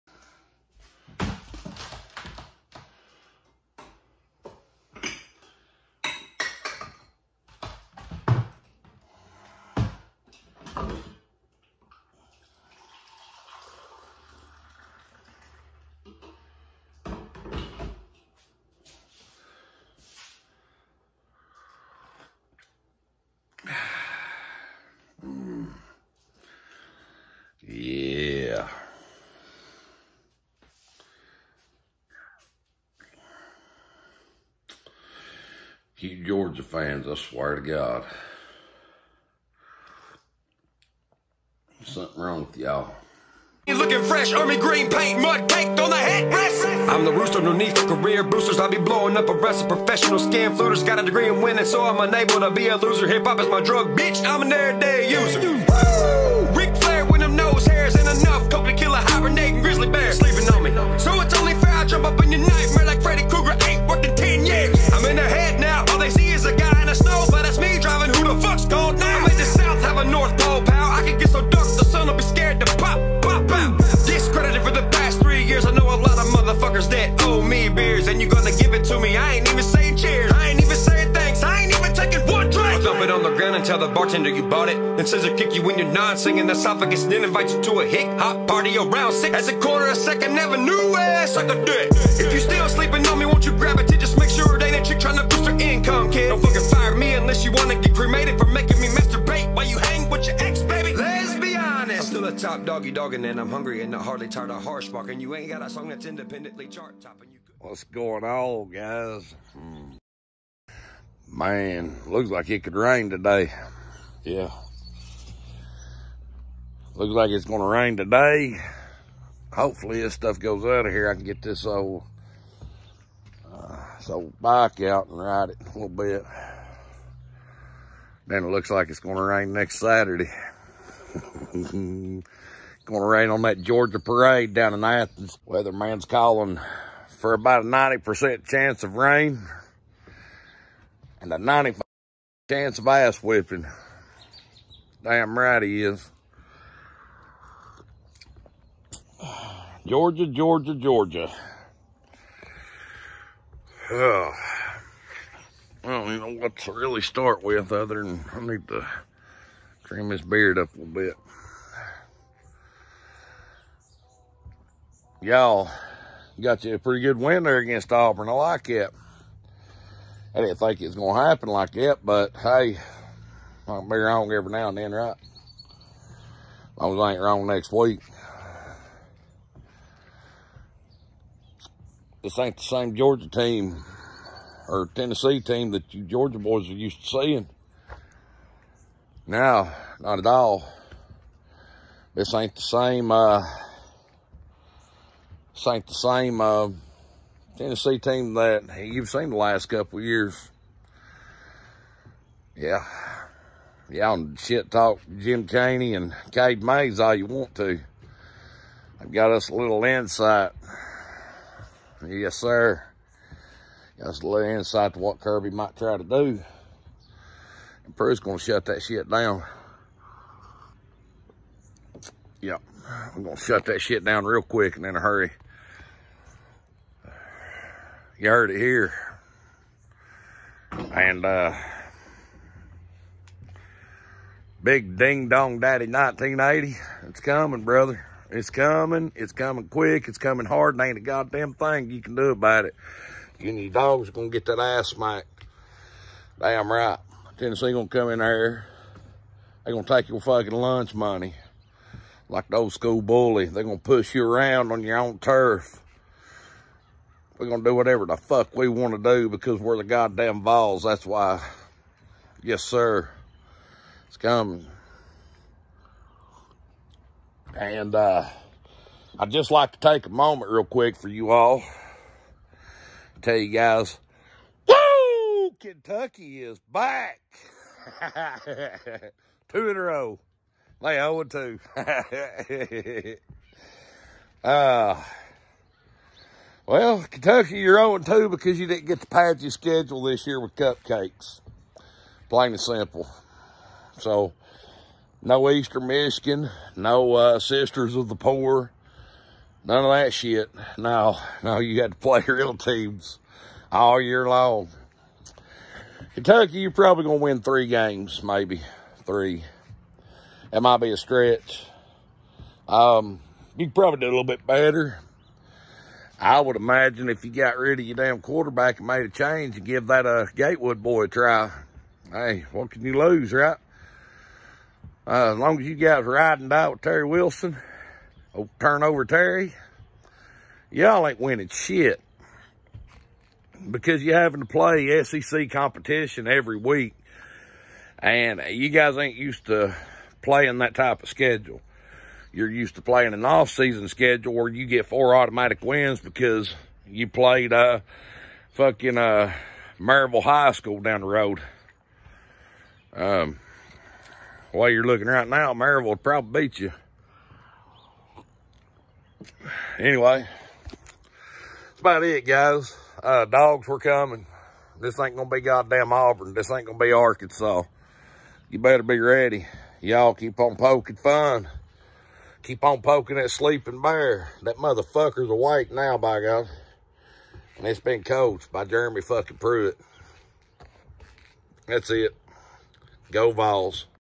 Georgia hell’s coming to Athens
Hells coming to Athens and the devil is wearing orange and white. Pruitt and the vols gonna trim those hedges and burn it done.